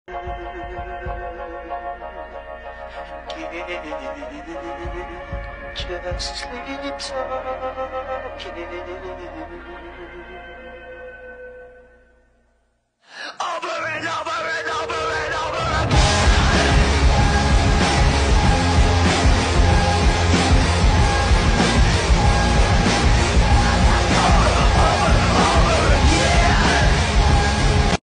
Камикадзе ВСУ по САУ Мста-С армии рф!
Kamikaze of the Ukrainian Armed Forces destroys the Msta-S self-propelled guns of the Russian army!